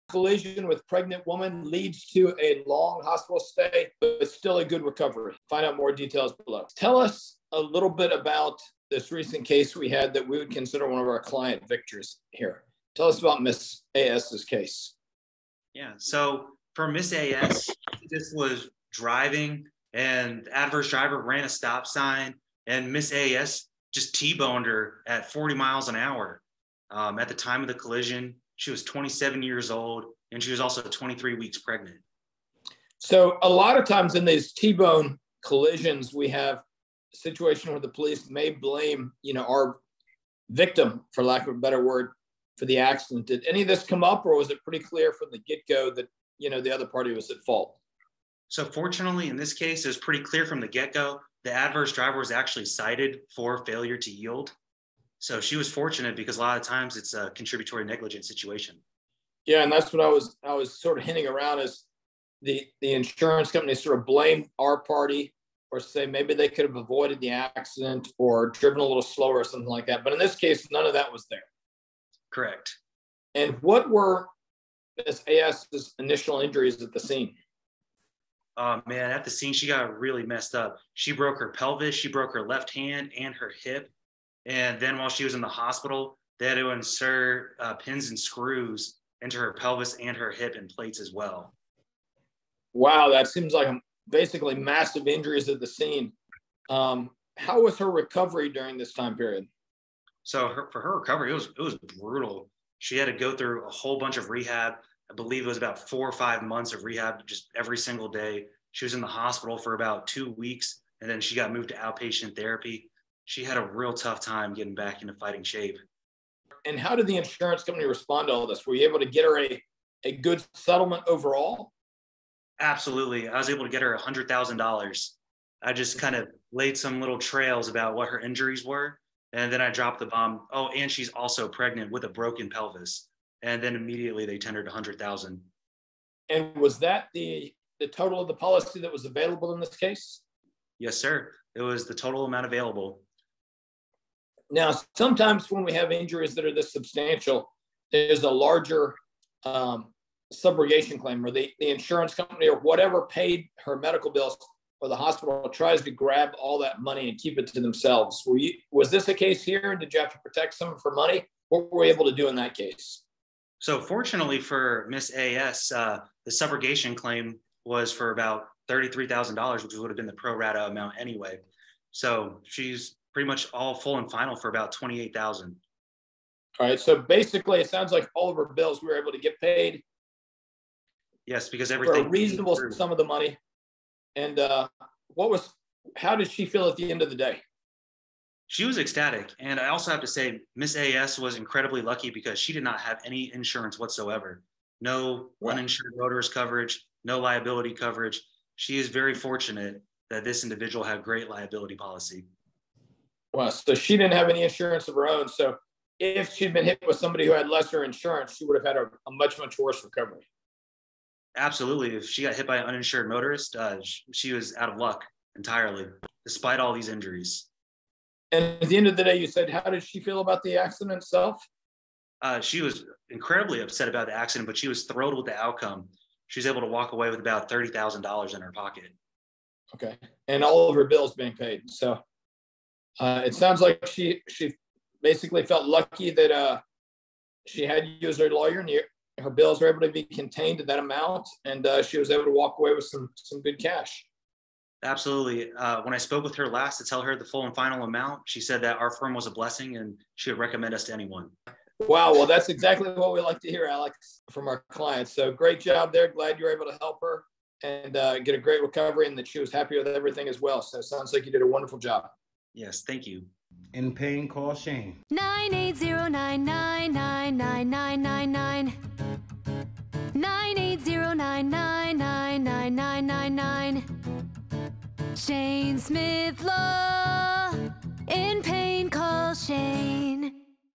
Collision with Pregnant Lady Leads To Long Hospital Stay - In Pain? Call Shane!
A severe collision breaks multiple bones in our pregnant client, who has to stay in the hospital for a long time to undergo various procedures! Alex was able to get her through a difficult recovery and get her a great settlement!

In Pain? Call Shane!
Sign up for our Weekly Newsletter for weekly chances to win trivia